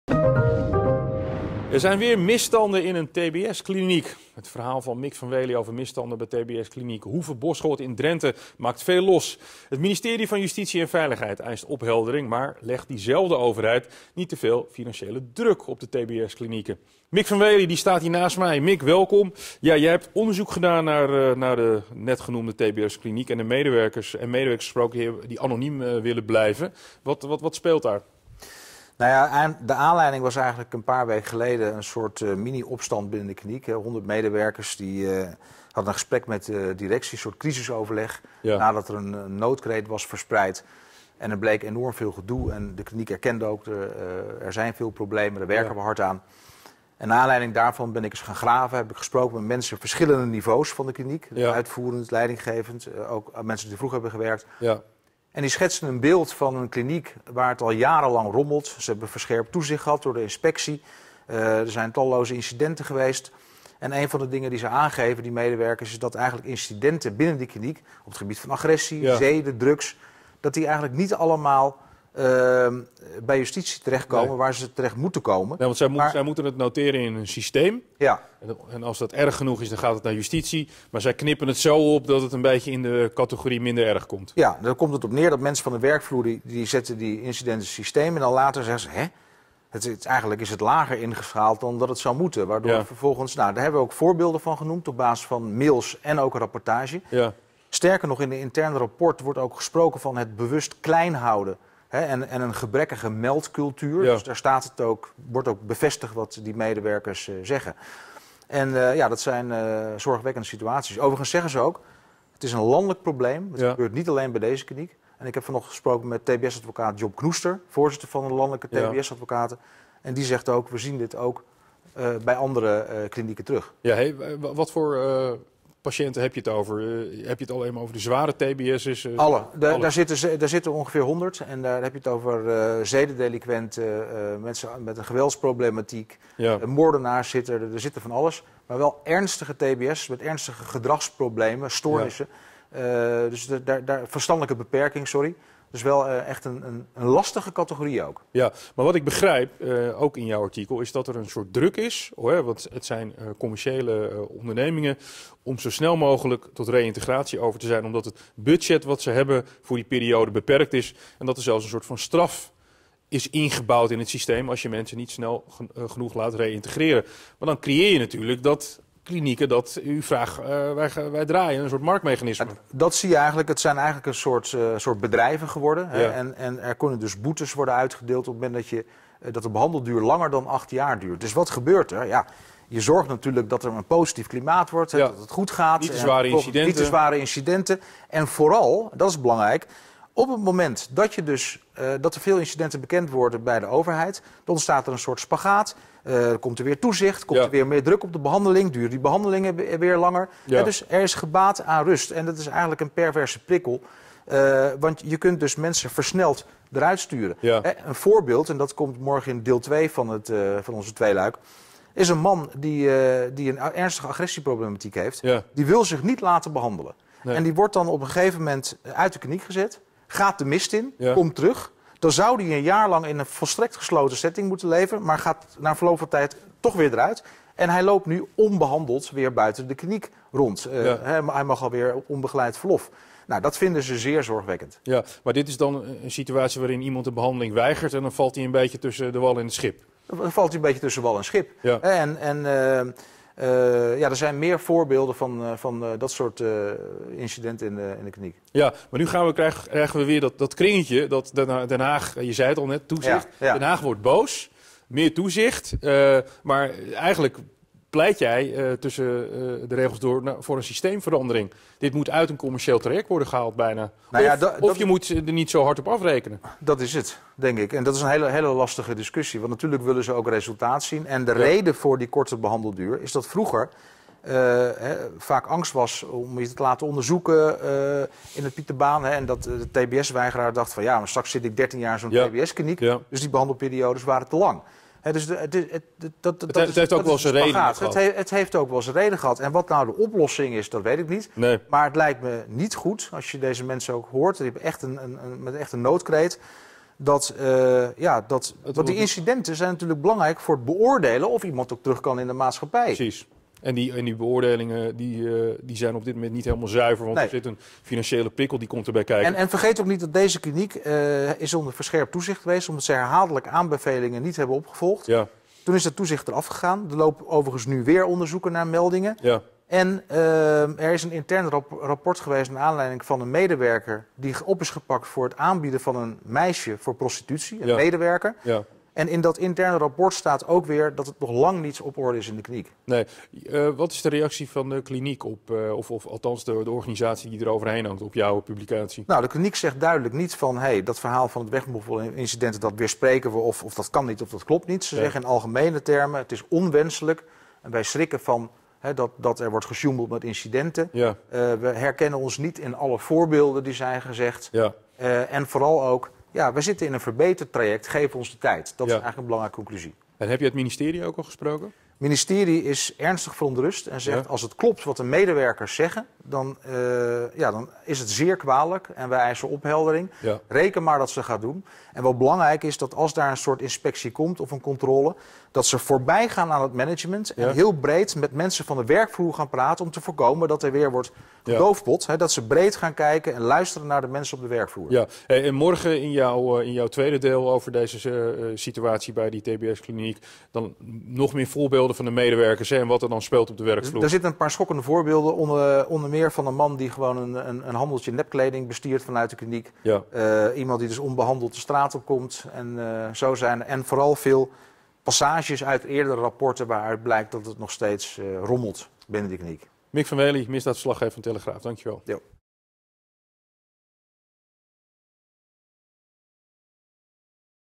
’Tbs-kliniek moffelt incidenten weg’
Er gaat van alles mis bij tbs-kliniek Hoeve Boschoord in Drenthe. Misdaadverslaggever Mick van Wely over het achterliggende systeem dat ervoor zorgt dat tbs-klinieken in een spagaat zitten.